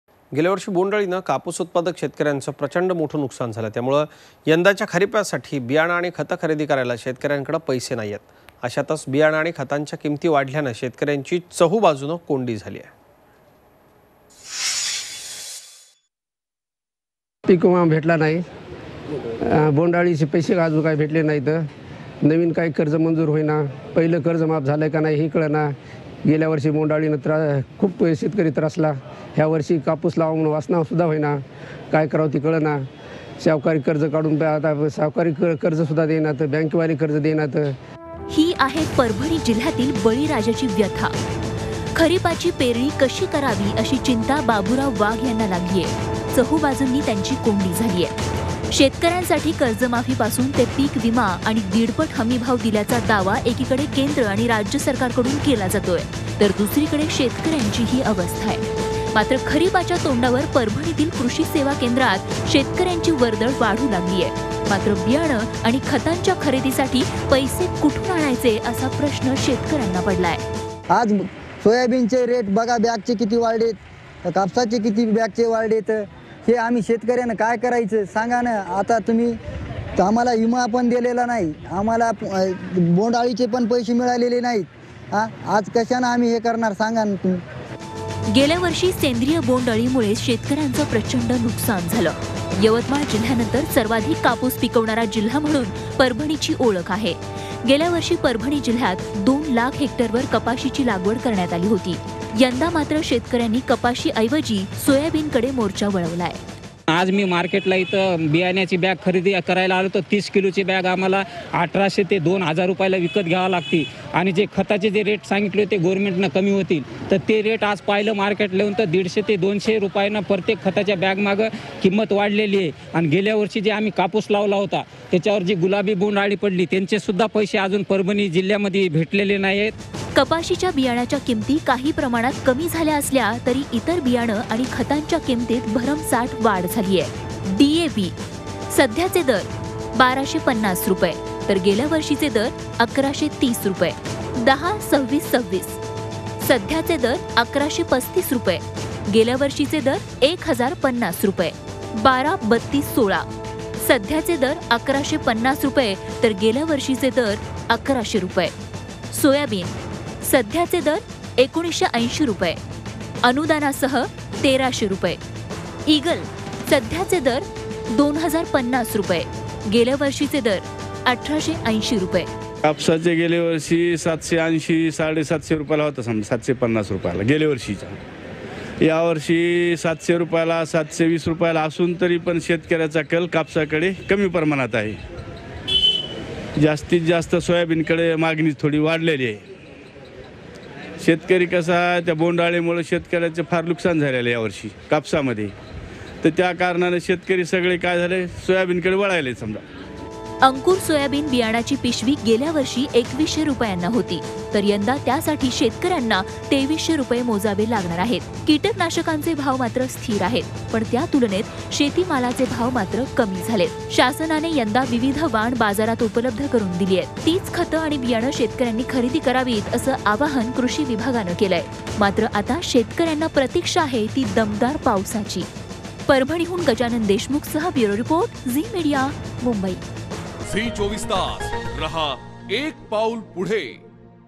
परभणी | शेतकऱ्यांची चहूबाजुने कोंडी
Parbhani | Farmers In Problem As No Money To Buy Seeds And Fertilizers